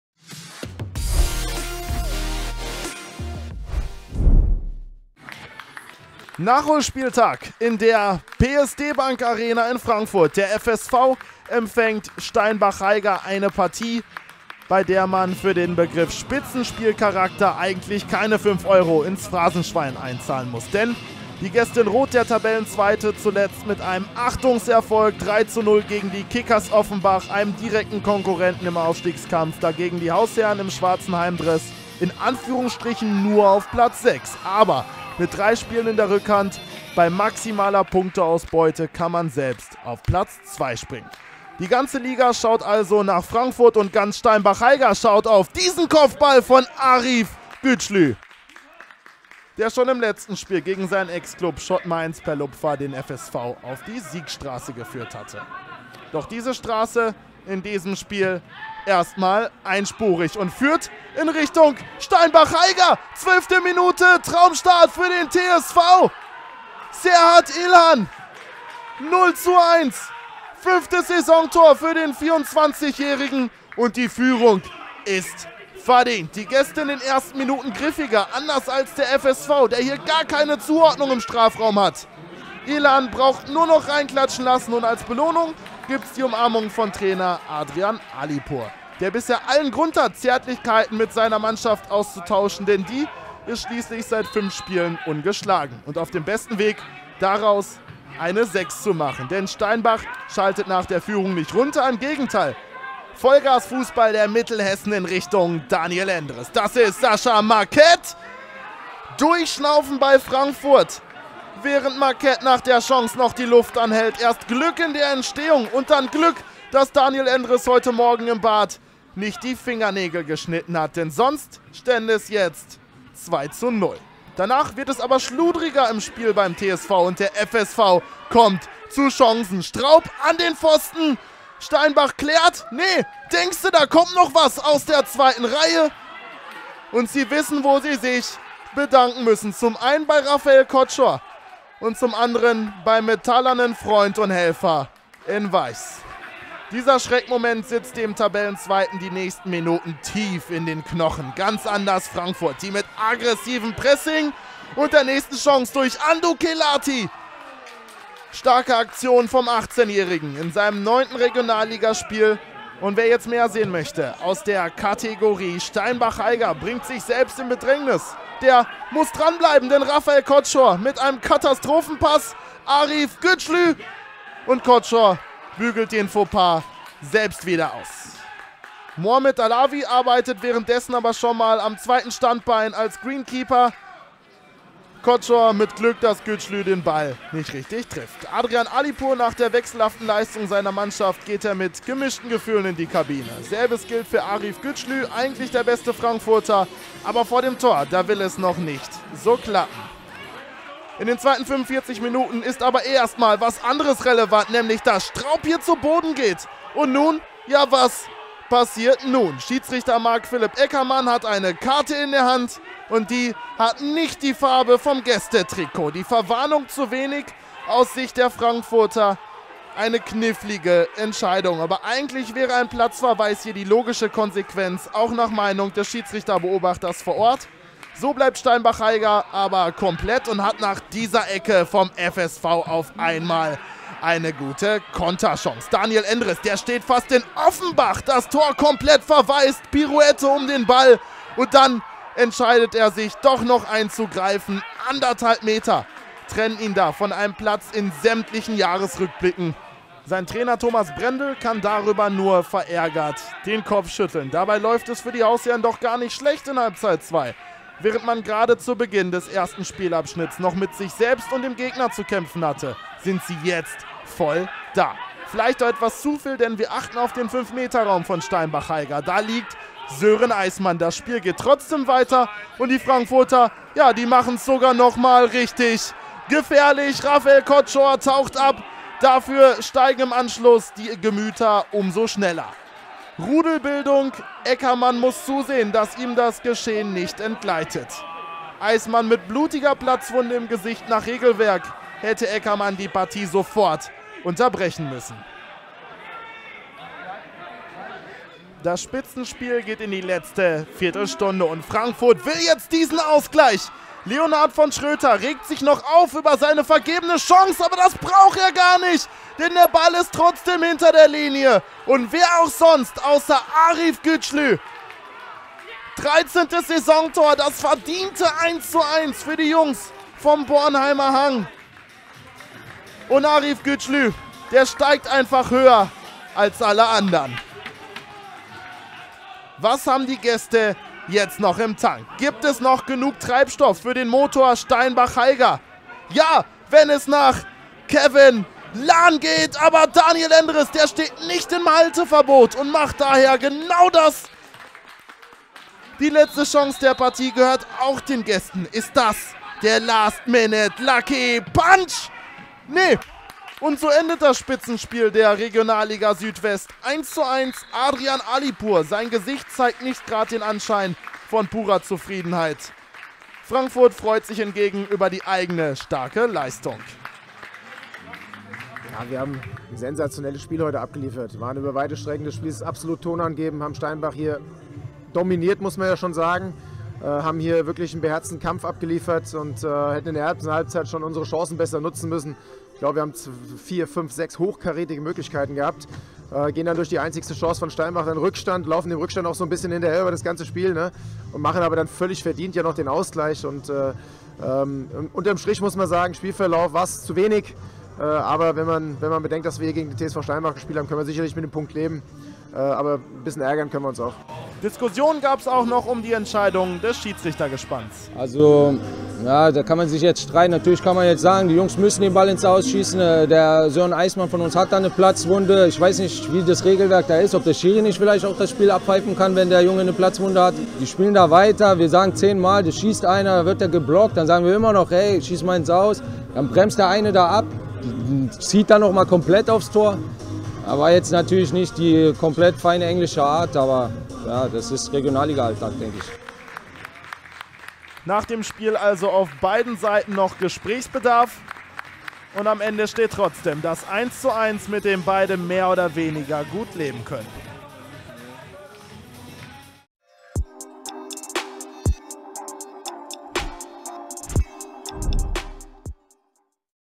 Hitziges Topspiel im Aufstiegsrennen | FSV Frankfurt - TSV Steinbach Haiger (Regionalliga Südwest)
Punkteteilung im Spitzenspiel!

21. Spieltag, 16.03.21

Tore:
0:1 Serhat Ilhan
1:1 Arif Güclü

Aufstellung FSV Frankfurt:
16 Daniel Endres, 4 Jesse- Garon Sierck, 7 Ahmed Azaouagh, 9 Arif Güclü, 14 Andu Yobel Kelati, 17 Steffen Straub, 19 Muhamed Alawie, 28 Leonhard von Schroetter, 29 Dominik Nothnagel, 30 Denis Mangafic, 32 Luca Bazzoli

Ersatzbank:
1 Kenan Mujezinovic, 6 Fabian Burdenski, 11 Mischa Häuser, 12 Marco Speck, 18 Jake Hirst, 20 Emir Sejdovic, 24 Canel Burcu

Trainer:
Thomas Brendel

Aufstellung TSV Steinbach Haiger:
1 Raphael Koczor, 2 Ivan Mihaljevic, 5 Sasa Strujic, 6 Christian März, 8 Dino Bisanovic, 11 Florian Bichler, 14 Philipp Hanke, 17 Sascha Marquet, 20 Serhat Ilhan, 28 Benjamin Kirchhoff, 31 Sören Eismann

Ersatzbank:
33 Johann Hipper, 3 Ko Sawada, 4 Maurice Buckesfeld, 12 Gian Maria Olizzo, 15 David Haidar, 18 Kevin lahn, 26 Enis Bytyqi

Trainer:
Adrian Alipour

Kommentar: Julian-Luca Schäfer
Produktion: Valentin Pfeifer